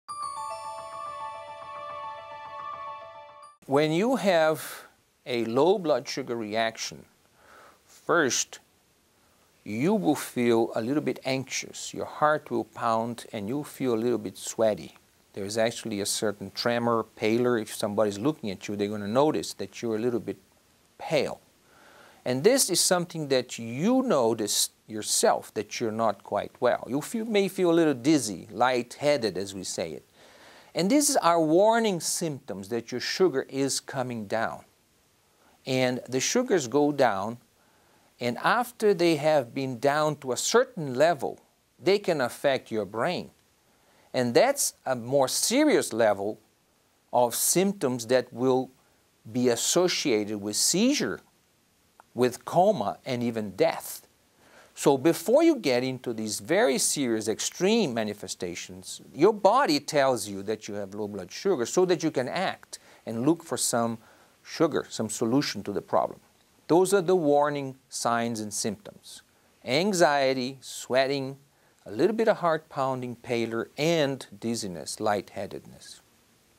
What Are the Symptoms of Diabetic Low Blood Sugar Reaction?
Learn about diabetes low blood sugar reaction symptoms. MDs answer your diabetes questions.